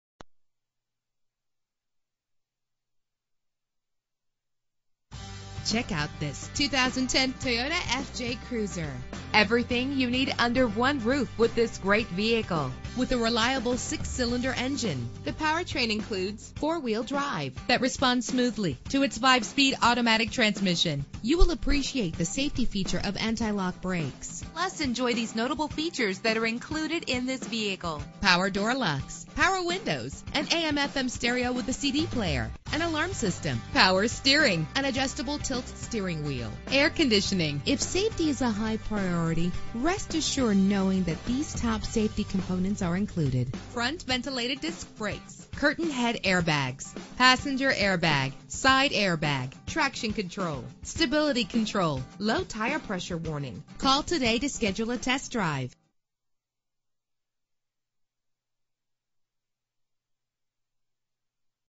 2010 Toyota FJ Cruiser - Las Vegas NV
Year: 2010
Make: Toyota
Model: FJ Cruiser
Trim:
Engine: 4.0 liter 6 cylinder 24 valve
Transmission: 5-Speed Automatic
Color: Black
Mileage: 51500
Address:
4956 Paradise Rd.